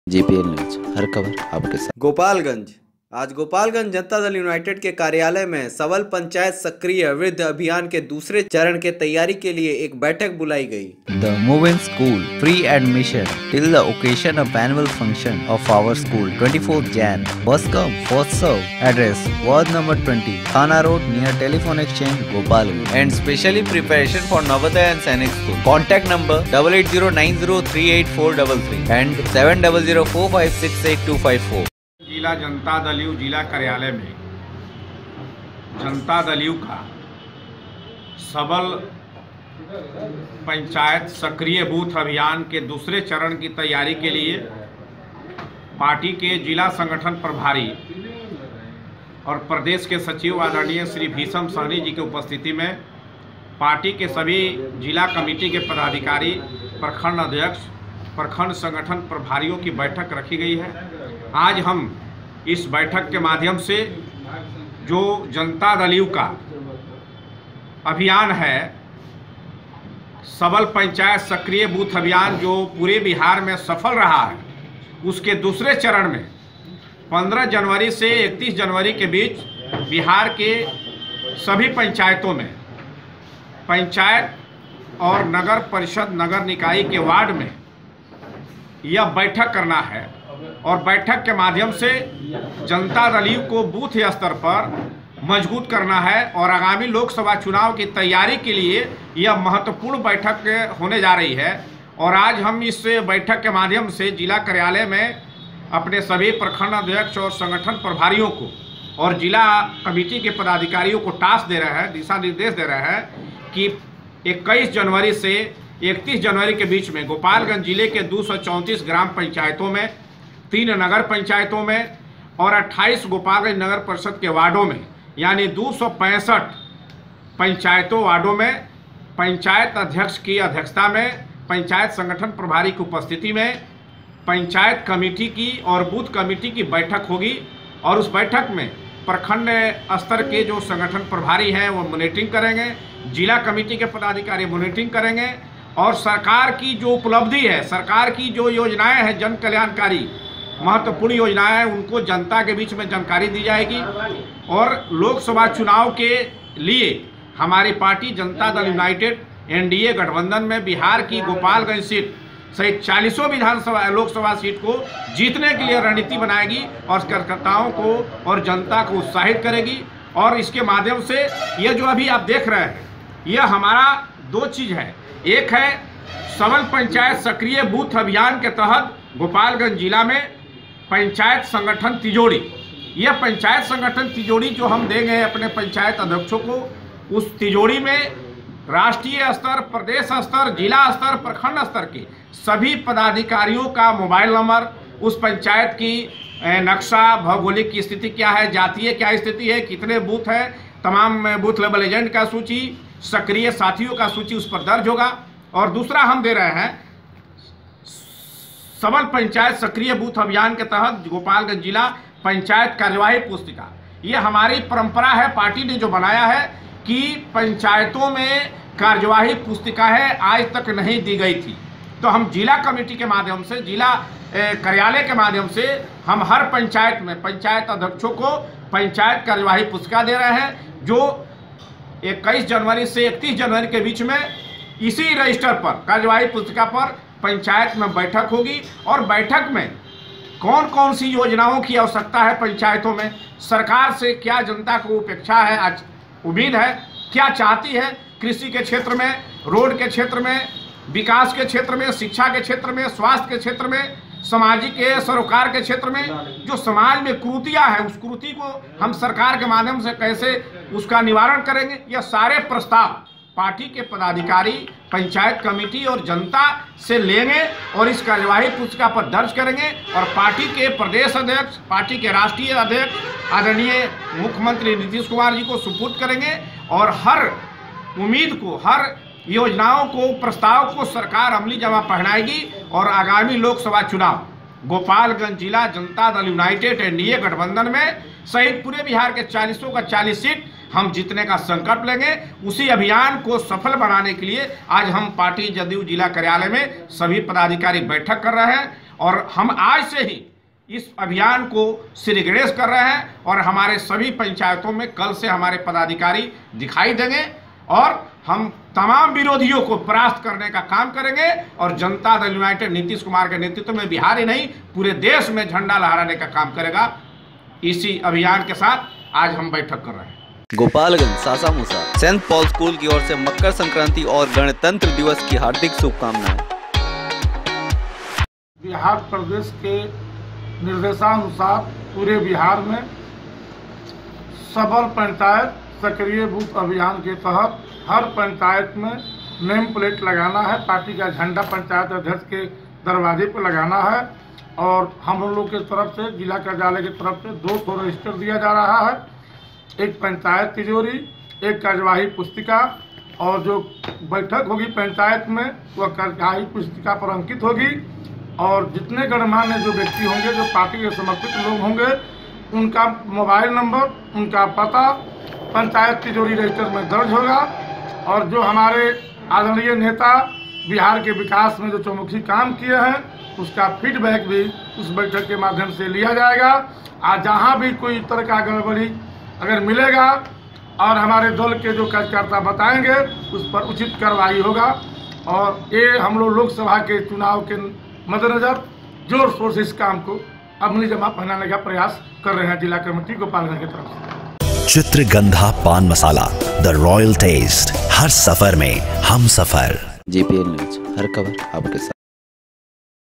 दूसरे चरण के तैयारी के लिए एक बैठक बुलाई गई Gopalganj News(GPL NEWS)
आज गोपालगंज जनता दल यूनाइटेड के कार्यालय में सबल पंचायत सक्रिय बूथ अभियान के दूसरे चरण के तैयारी के लिए एक बैठक बुलाई गई